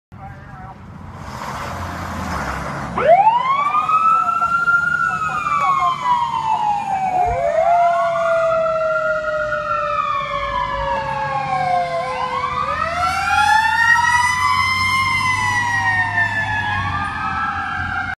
Engine 532 responding
Responding to mutual aid to Amanda Township Fire 520 for a working Structure Fire